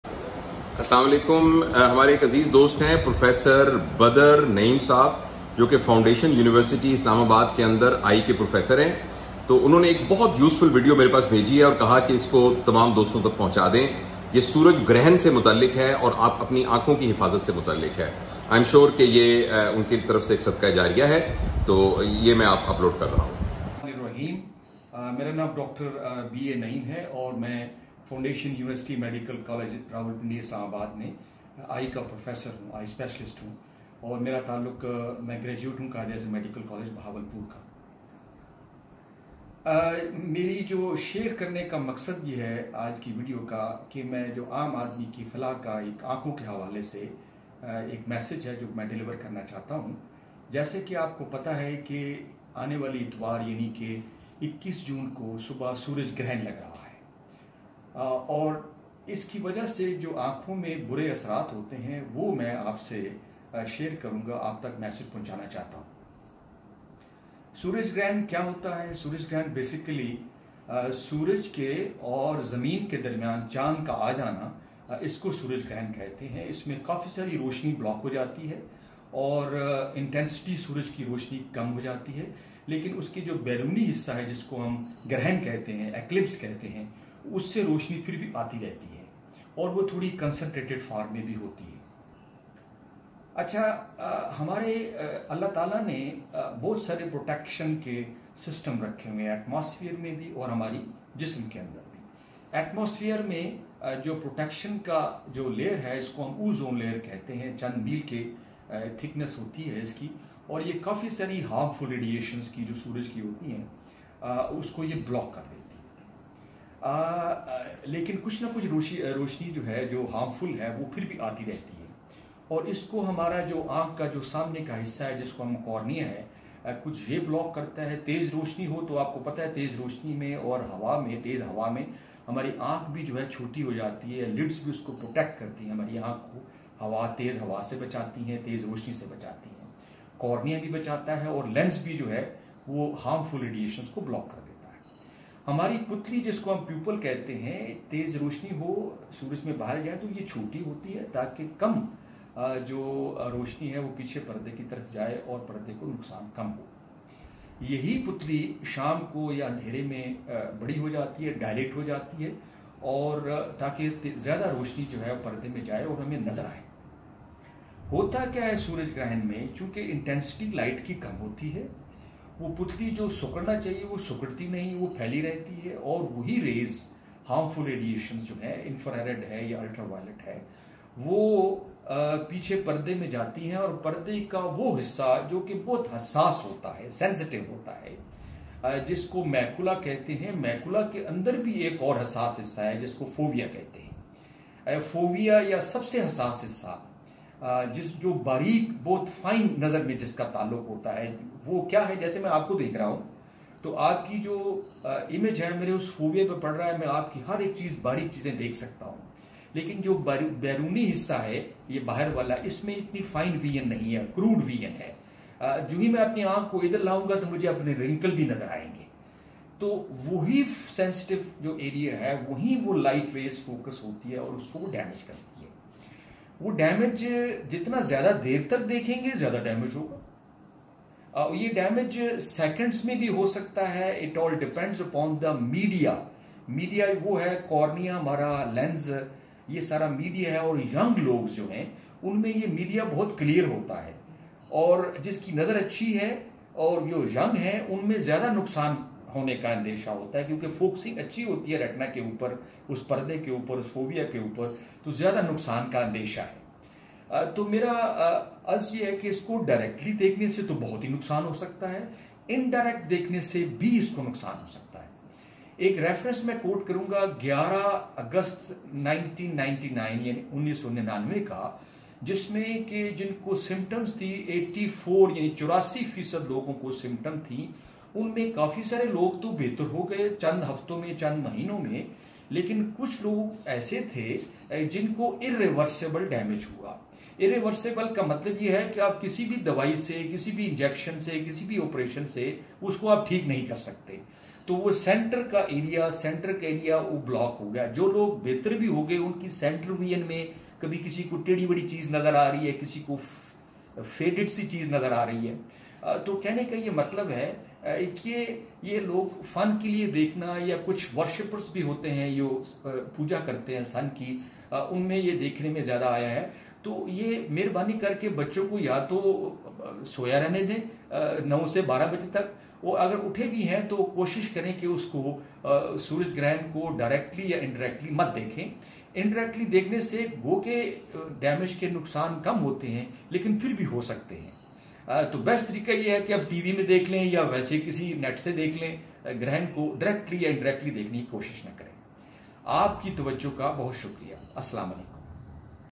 Sun eclipse and our eyes: |Urdu|prof dr javed iqbal
Prof Badar Naeem talking about effects of sun eclipse on eyes. A public service message .